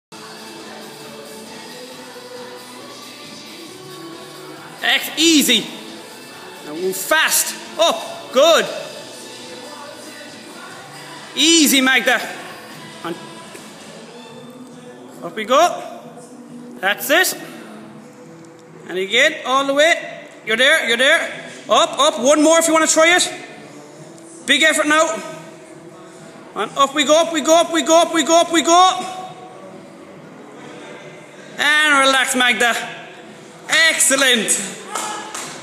Magda hit s her goal of 5 chin ups
Magdas 1st test in our April 2015 testing week.

Her goal was to hit 5 chin ups and she done it in style and almost got  a 6th

Well done Magda. Another example of hard, consistent and smart training.